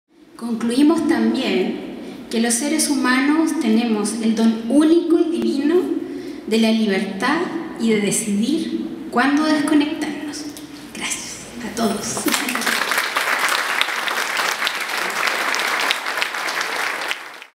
Cristina Vera Aguilar- Vive En Off
Instalación de Arte (Vive en Off) (Faltó memoria para seguir grabando disculpas y gracias)
8 al 21 de Octubre en Marina del Sol - Concepción
Cristina Vera
Ana maría Hidalgo
Yonarev Okanara